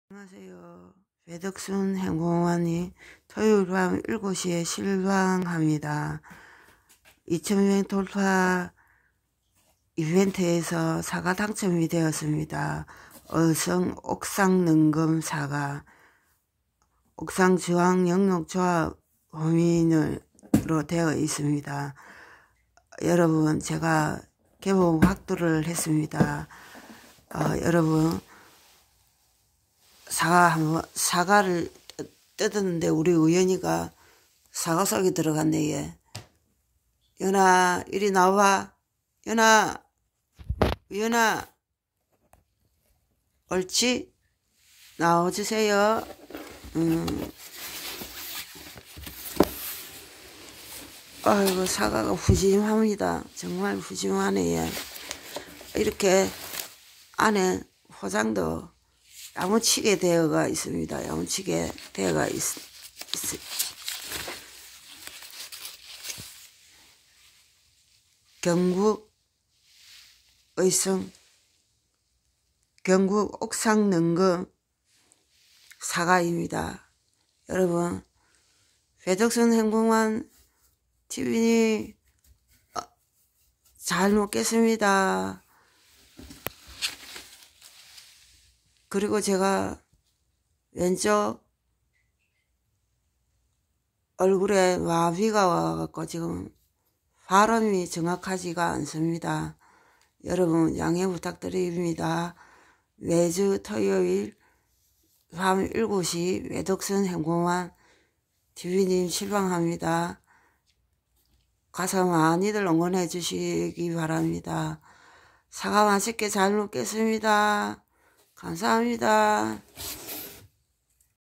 감사합니다#
안녕하세요 배덕순행복맘님 이벤트사과 꼼꼼하게 포장 택배감사합니다 행복한 하루 되세요